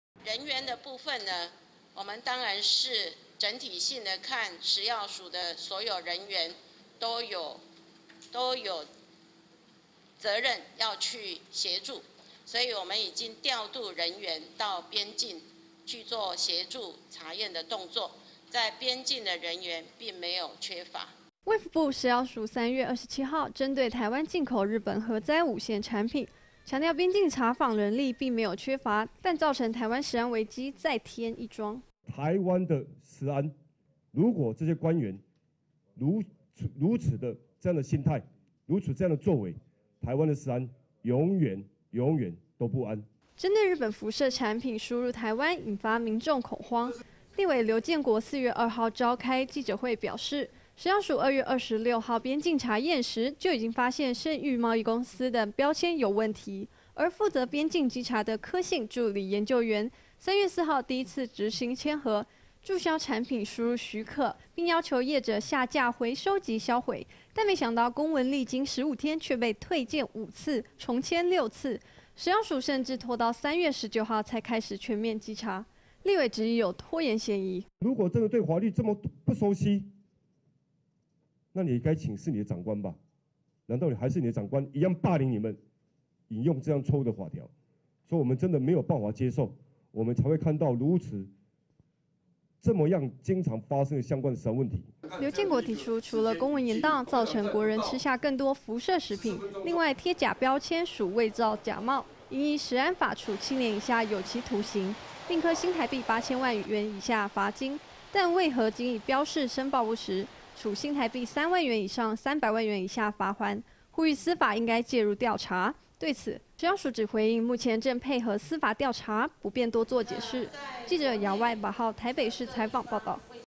核災區食品侵台 食藥署遭質包庇 2015-04-02 TITV 原視新聞
衛福部食藥署3月27號針對台灣進口日本核災5縣產品，強調邊境查訪人力並沒有缺乏，但造成台灣食安危機再添一樁。

針對日本輻射產品輸入台灣，引發民眾恐慌，立委劉建國4月2號召開記者會表示，食藥署2月26號邊境查驗時，就已經發現盛裕貿易公司標籤有問題，而負責邊境稽查的柯姓助理研究員，3月4號第一次執行簽核，註銷產品輸入許可，並要求業者下架回收及銷毀，但沒想到公文歷經15天卻被退件5次，重簽6次，食藥署甚至拖到3月19號才開始全面稽查，立委質疑有拖延嫌疑。

劉建國提出，除了公文延宕造成國人吃下更多輻射食品，另外，貼假標籤屬偽造假冒，應依食安法處7年以下有期徒刑，併科新臺幣8千萬元以下罰金，但為何僅以標示申報不實，處新台幣3萬元以上300萬元以下罰鍰，呼籲司法應該介入調查。對此，食藥署只回應，目前正配合司法調查，不便多做解釋。